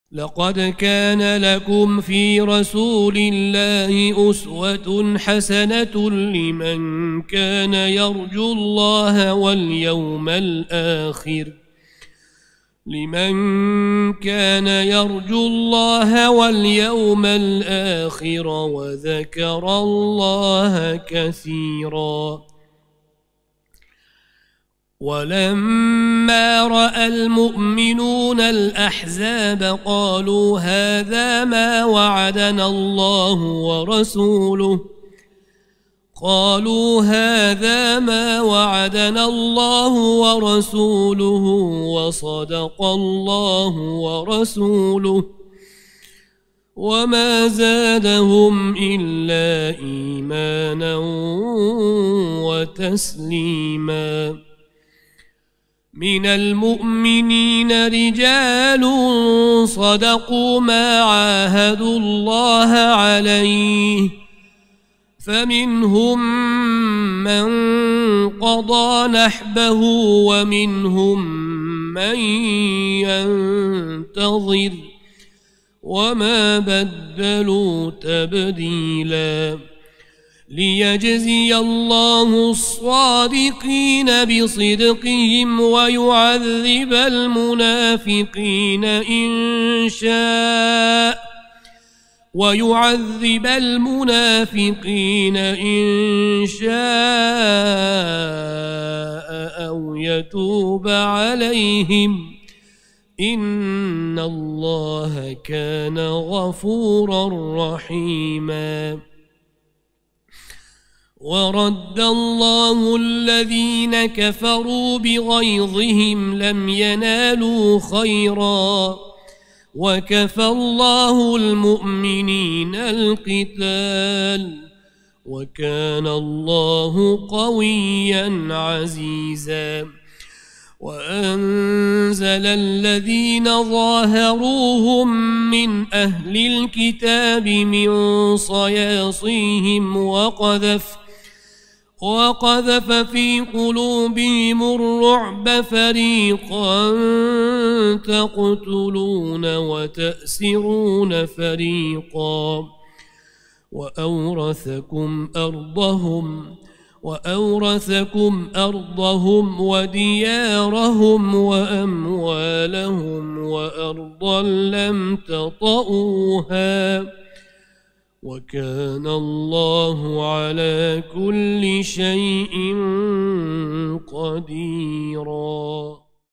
Ahzob surasi 21-oyatdan 27-oyatgacha | Hasanxon Yahyo Abdulmajid | Taroveh namozi 17-kun
Shayx Muhammad Sodiq Muhammad Yusuf» jome' masjidi imom noibi Hasanxon Yahyo Abdulmajid

Ramazon 1444 | 2023